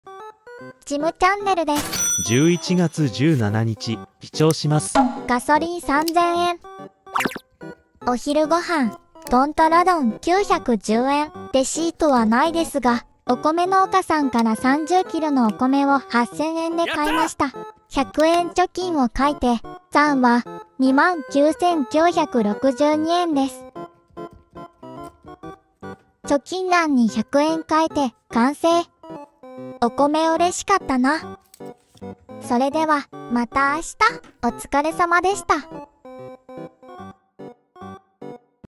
第22章11月17日、お米安く買えました！🌾生活費1ヶ月10万円の行方を記載していきます！貯金 #家計簿
貯金は楽しくやりましょう#貯金のコツ

1日1000円貯金チャレンジ365日目！目指せ〇〇万、1年間ありがとうございました！達成！